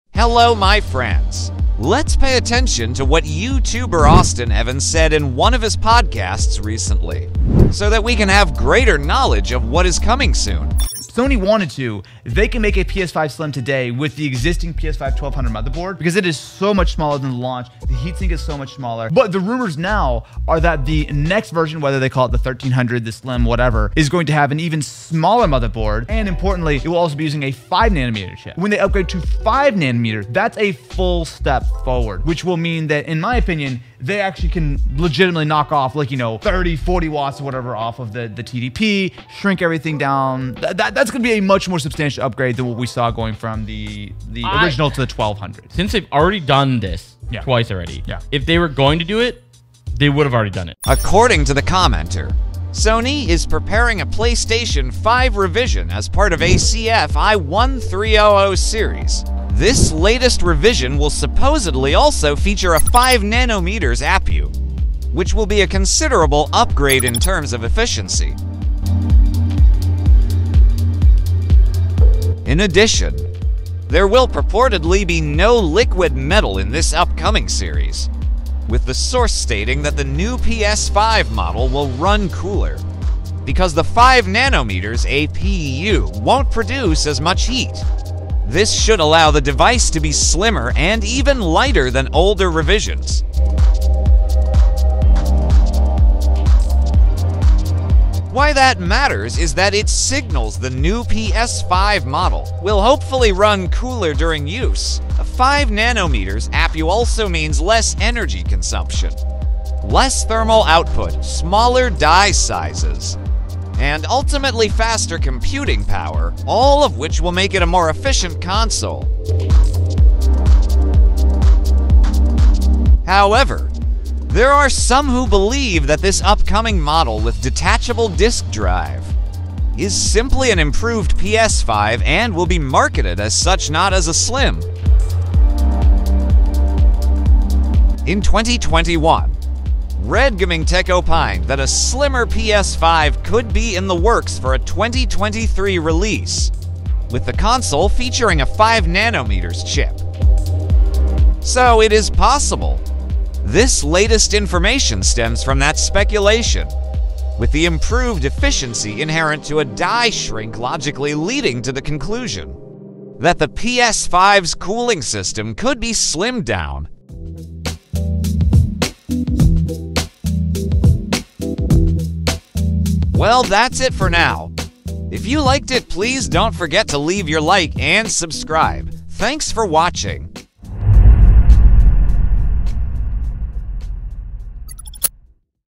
🔥[PS5 SLIM] IT'S COMING... RELEASE DATE 2023... 5NM AND MORE !!!
According to the commenter, Sony is preparing a PlayStation 5 revision as part of a CFI-1300 series. This latest revision will supposedly also feature a 5 nm APU, which will be a considerable upgrade in terms of efficiency over the OG PS5’s custom 7 nm APU from AMD (a 6 nm revision was pushed out in 2022).